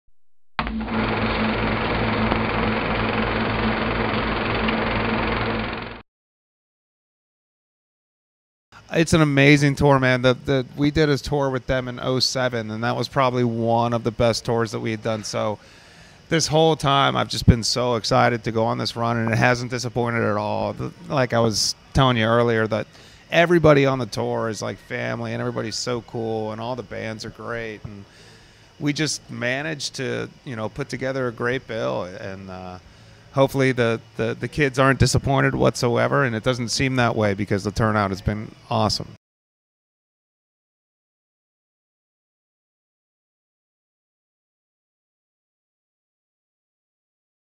LAMB OF GOD WILLIE ADLER TALKS TOURING WITH SLIPKNOT
Lamb Of God guitarist Willie Adler talks about touring with Slipknot in 2015.